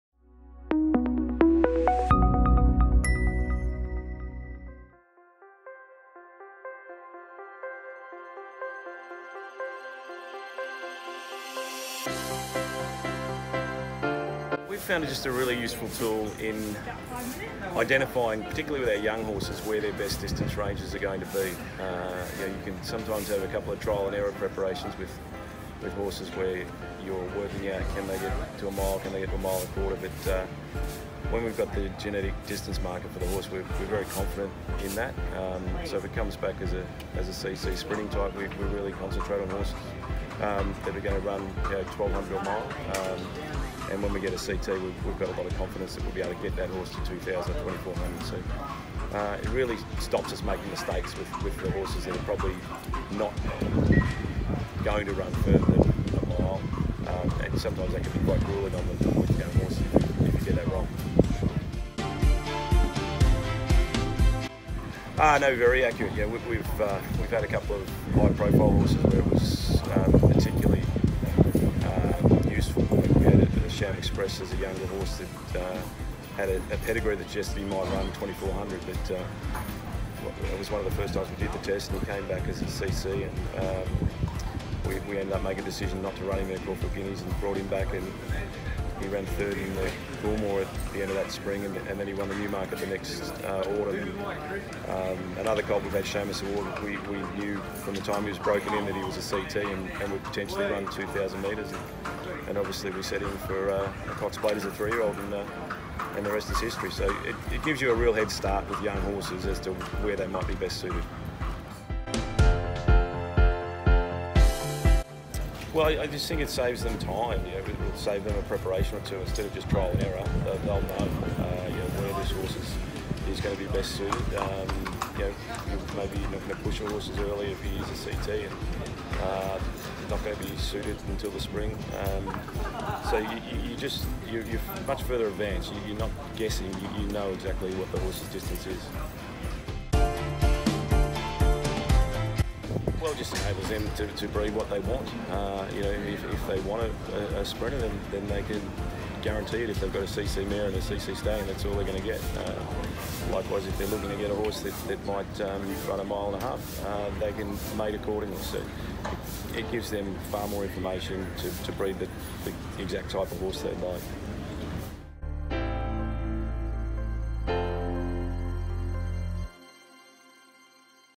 Danny O'Brien On Plusvital Genetic Testing
Watch multiple Group 1 winning Australian trainer Danny O’Brien give his thoughts on Plusvital genetic testing.